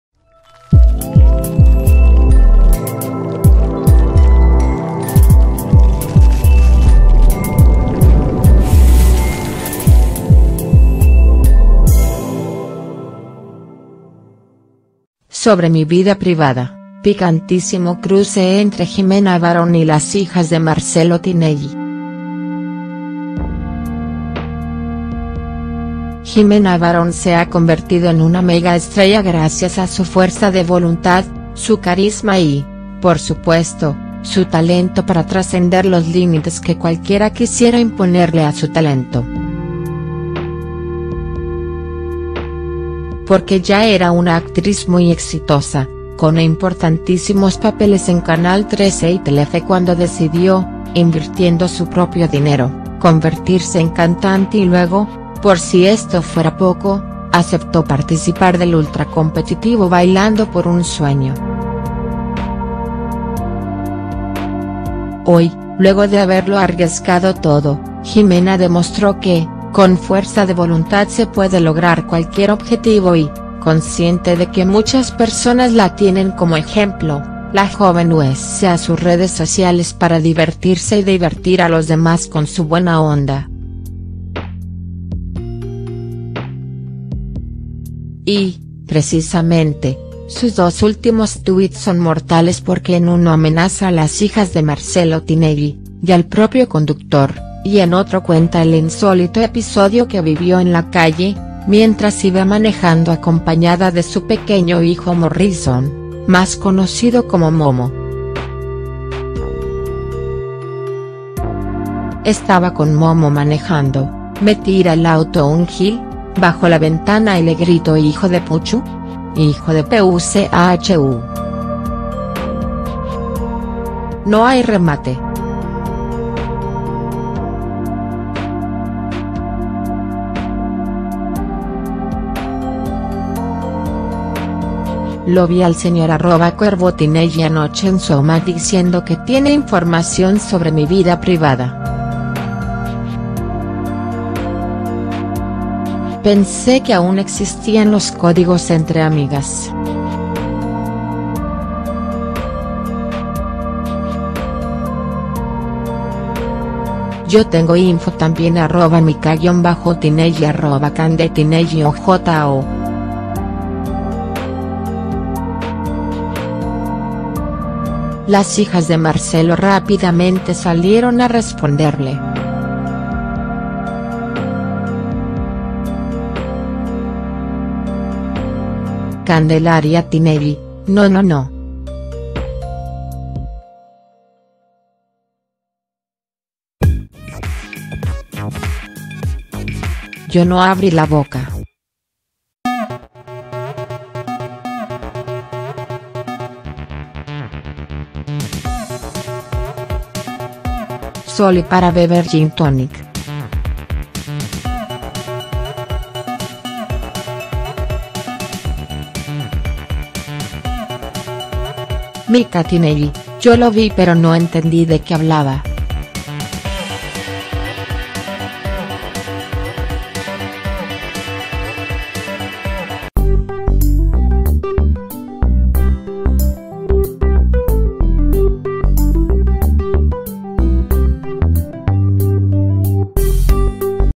"Sobre mi vida privada...": ¡Picantísimo cruce entre Jimena Barón y las hijas de Marcelo Tinelli!
La actriz fue muy sincera y eso provocó una rápida respuesta de Mica y Candelaría Tinelli. ¡Mirá!